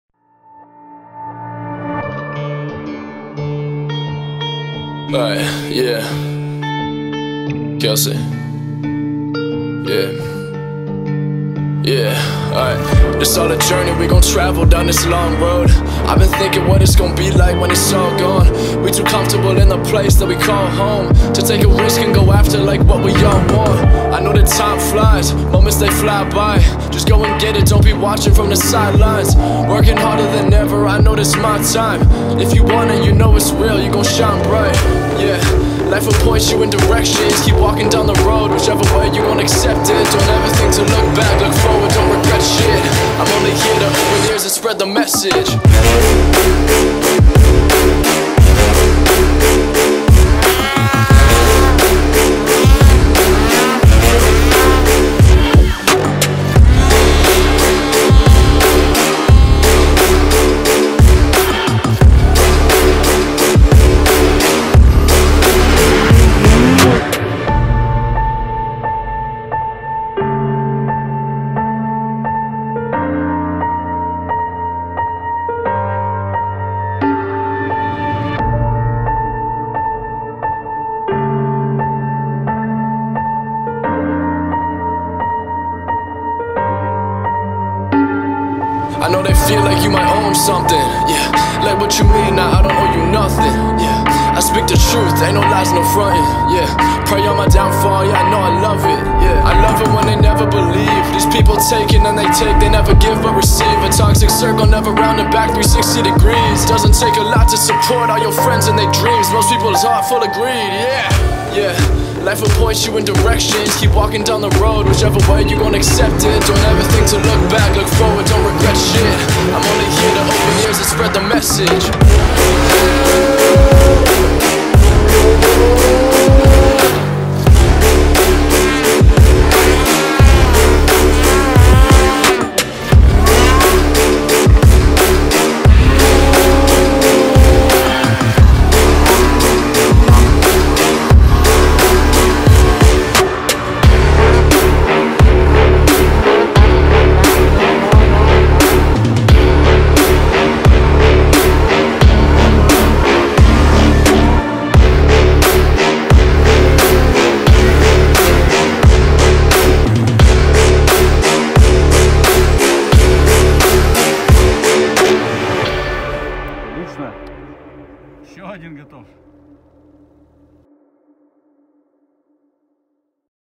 Гора Ослянка. Снегоходный сезон 2019-2020
Пилим горы: Ослянка / Одинокая / Камень Хариусный